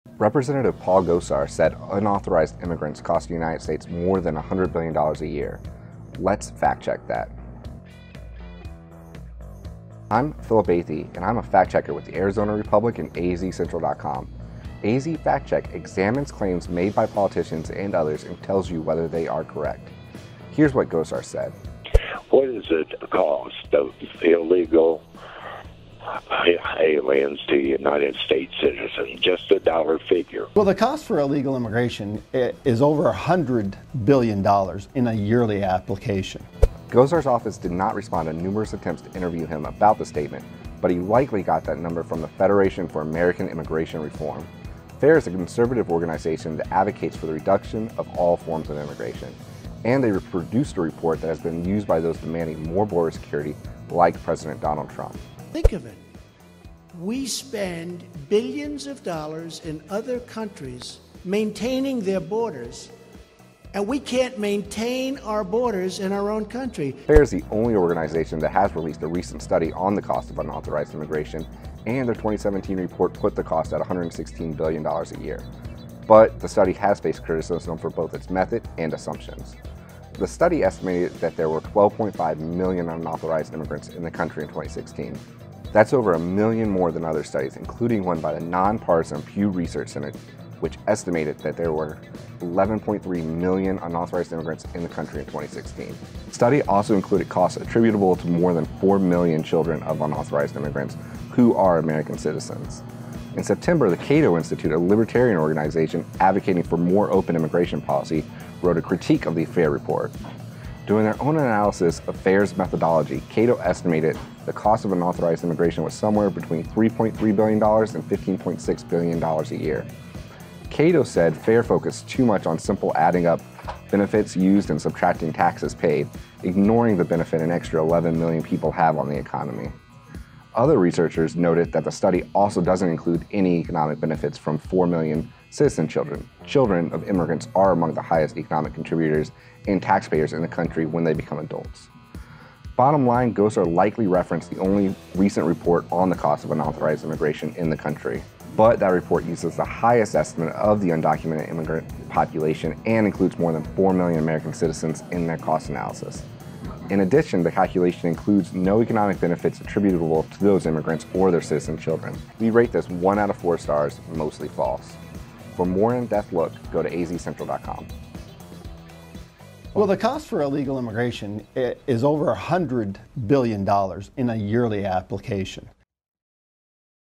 Does illegal immigration cost the United States more than $100 billion a year? - AZ Fact Check
Arizona Republican Rep. Paul Gosar said the cost of illegal immigration is over $100 billion a year. How accurate is that number? Read our thorough examination of the claim